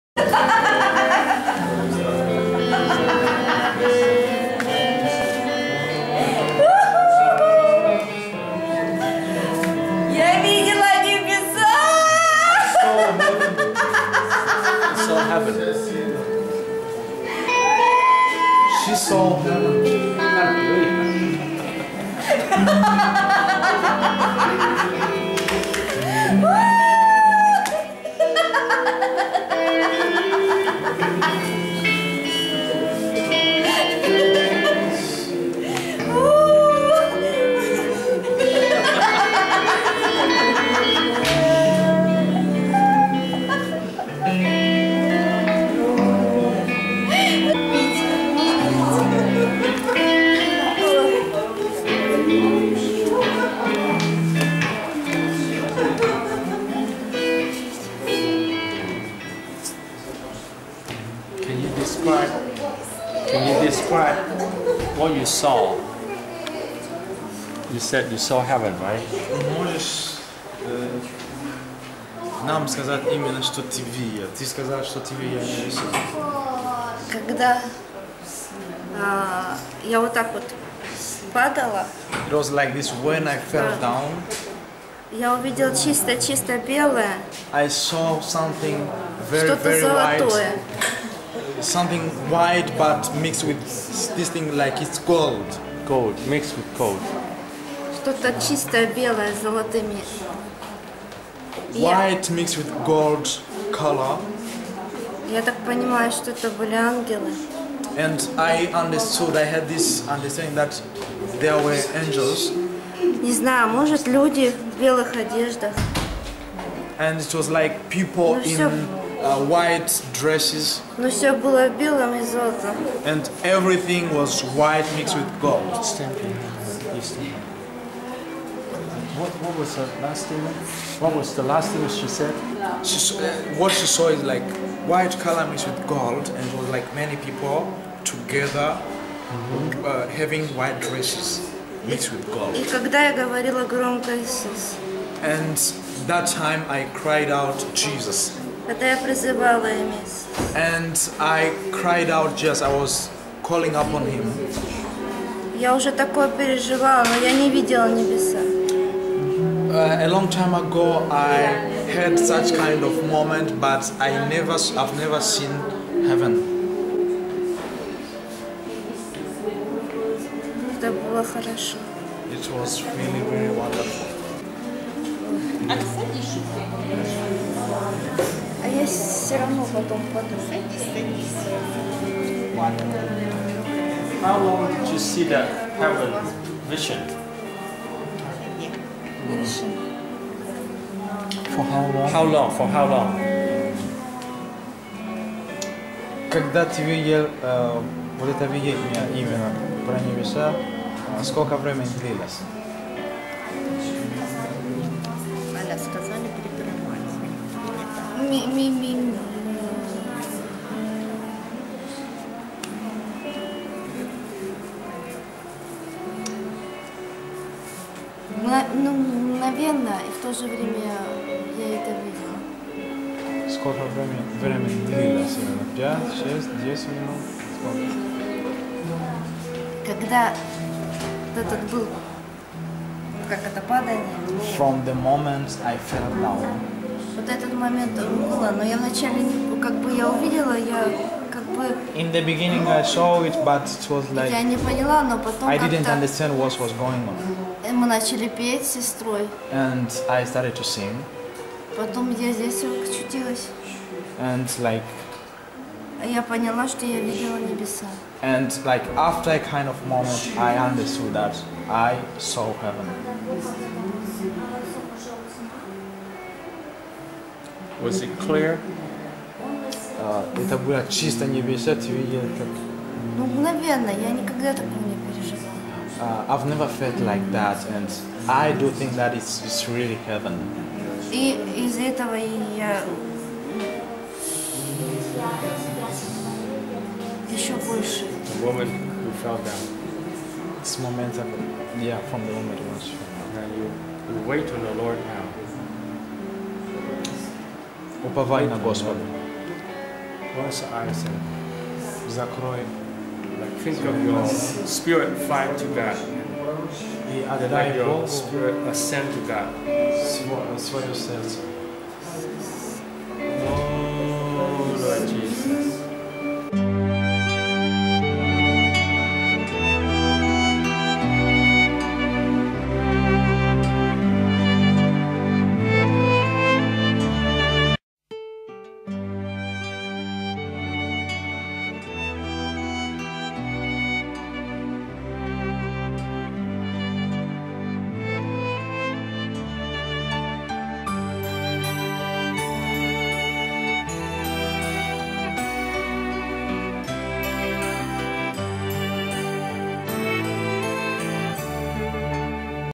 A woman was filled with joy & shared what she saw in heaven,Русский,in Russian,& English-Pastor Yip
A woman shared what she saw in heaven,Русский,in Russian,& English